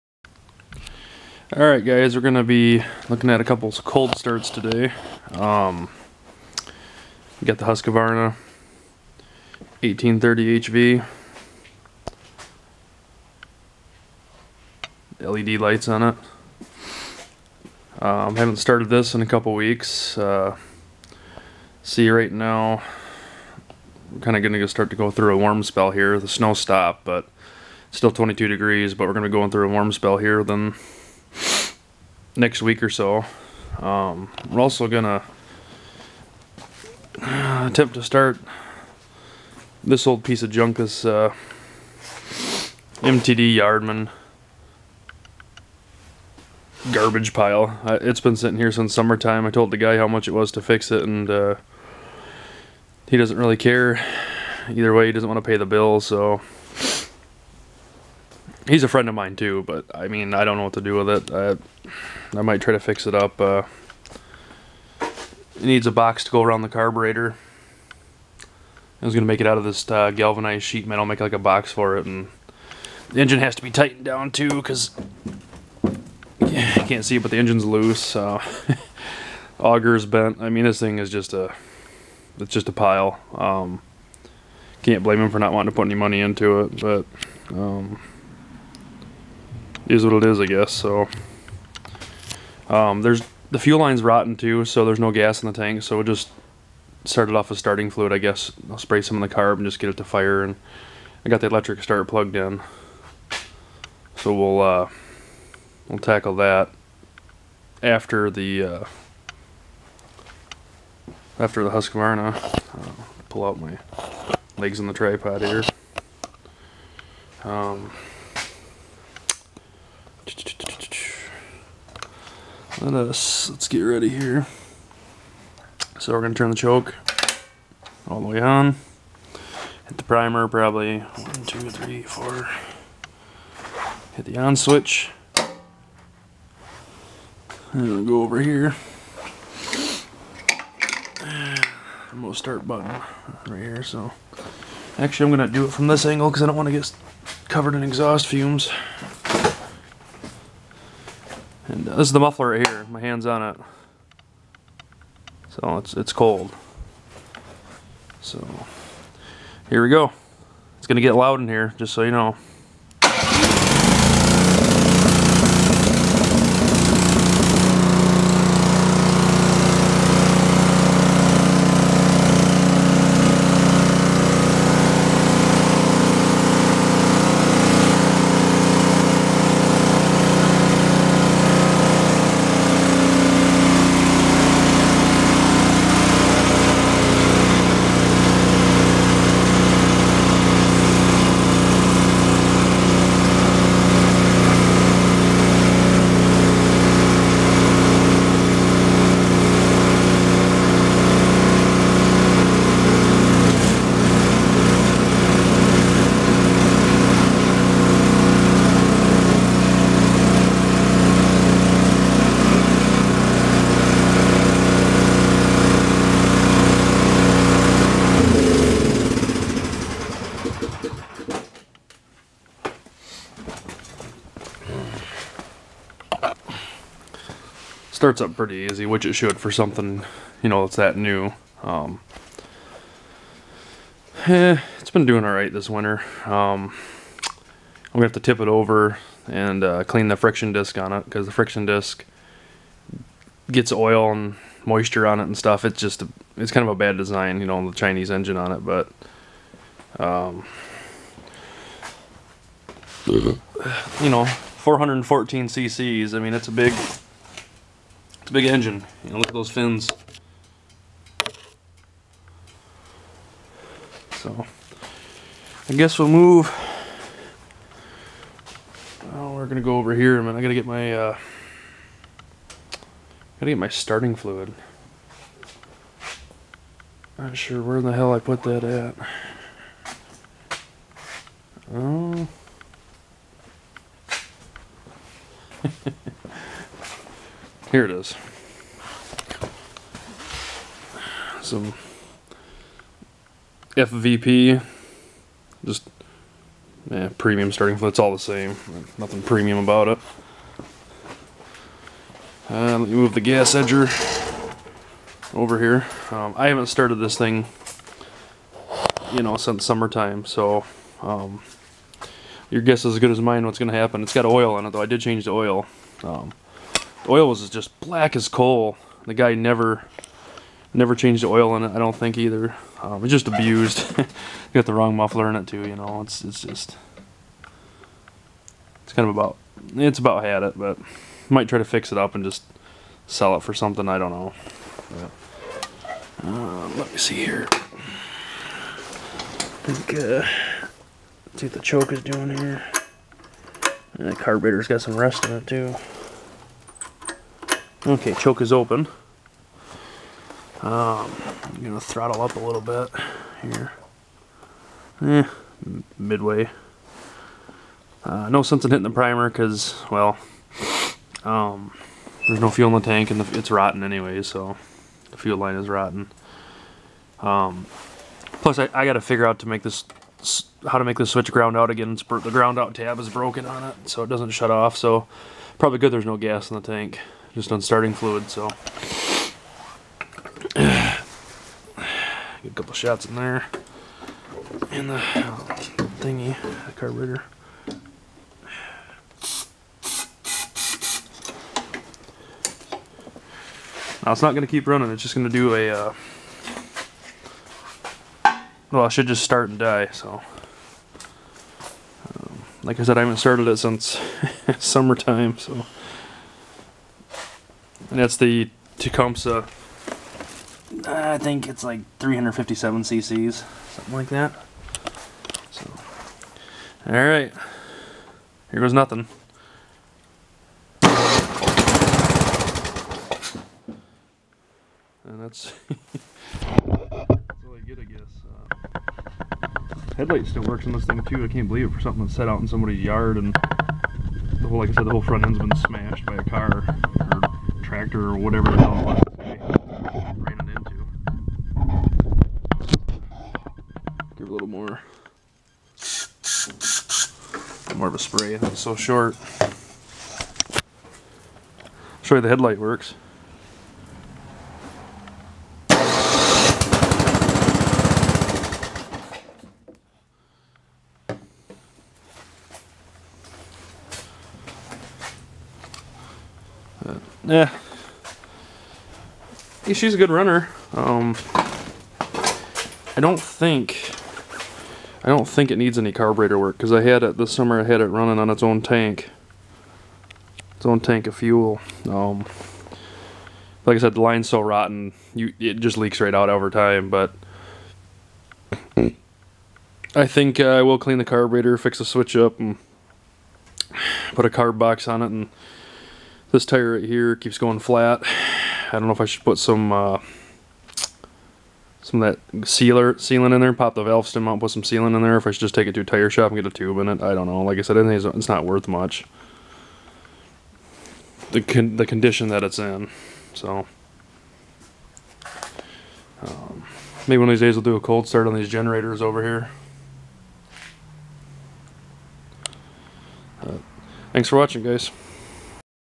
Husqvarna 1830HV Cold start with Yard Man Mtd 30 inch snow blower cold start
Was 22* out and started the newer husqvarna and old MTD snow blower,thrower.Mtd is just a junk spare time project.